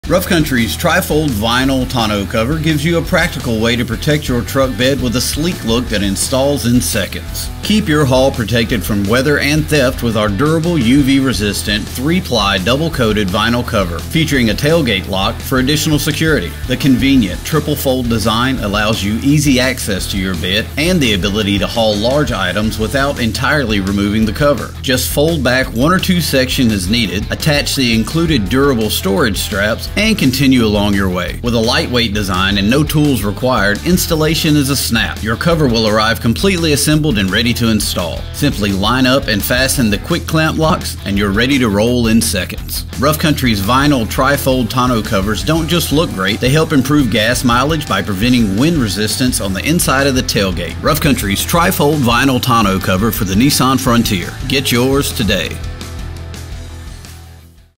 2005-2022 Nissan Frontier Soft Tri-Fold Bed Cover
Keep your haul protected from weather and theft with our durable UV resistant, 3 ply double coated vinyl cover featuring a tailgate lock for additional security. The convenient triple-fold design allows you easy access to your bed or the ability to haul large items without entirely removing the cover. Just fold back one or two sections as needed, and attach the included durable storage straps and continue along your way.